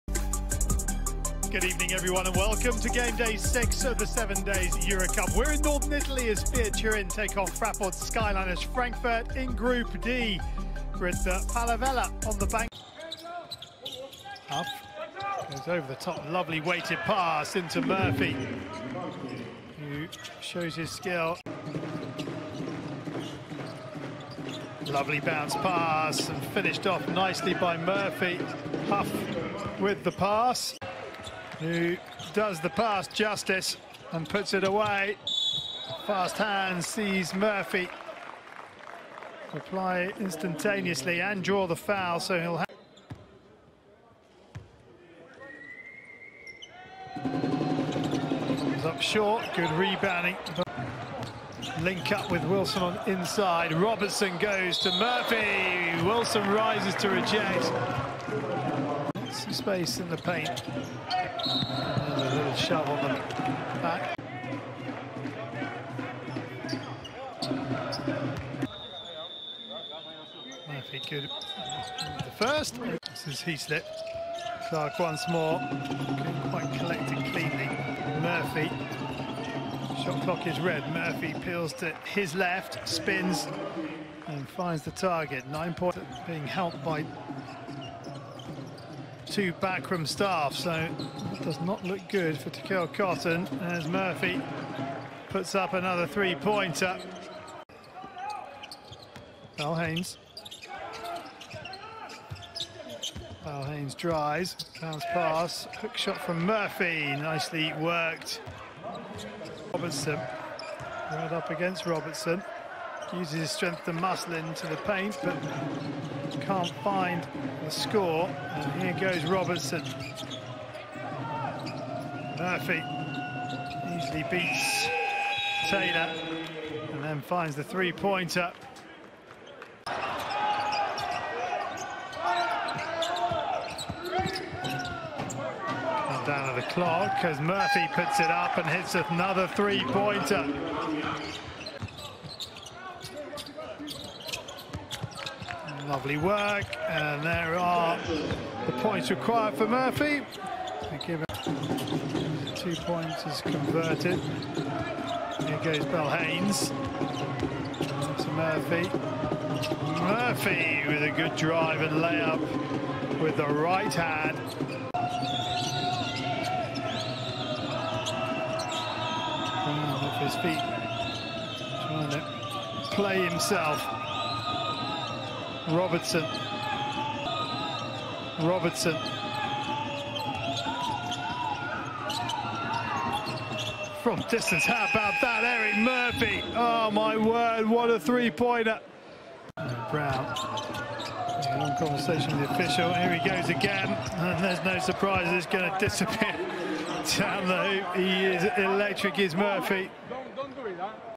Erik Murphy Full Highlights 06.11.2018. Fiat Turin vs Skyliners - 33 Pts, 7 Rebs! | UF44 Highlights
Erik Murphy Full Highlights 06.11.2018. Fiat Turin vs Skyliners - 33 Pts, 7 Rebs! | UF44 Highlights - Eurocup 2018/19
Turk Telekom 81 -  Zenit 75